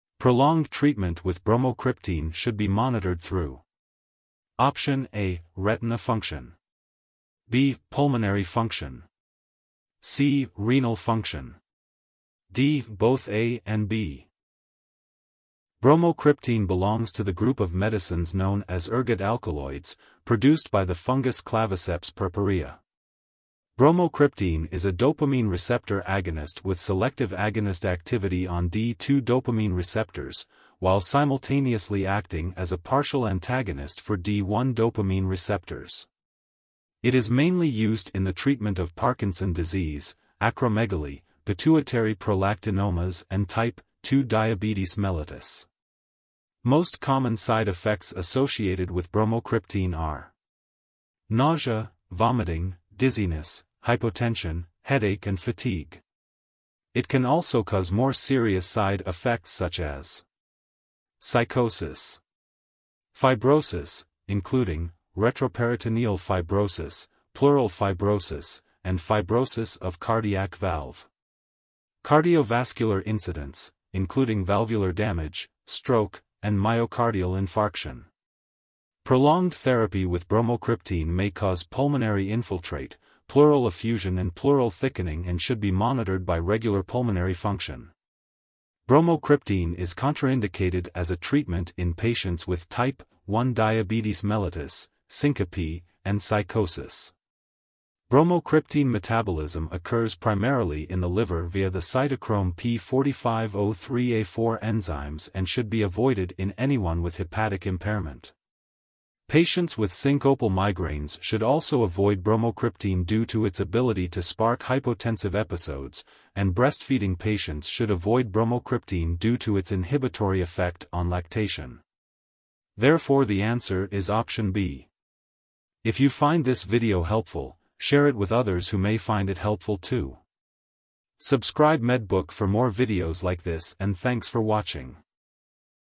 PEBC PHARMACIST EVALUATING EXAM QUESTIONS, PART 18
These videos will be helpful for, those who are preparing Canadian pharmacist licensing examination known as, PEBC.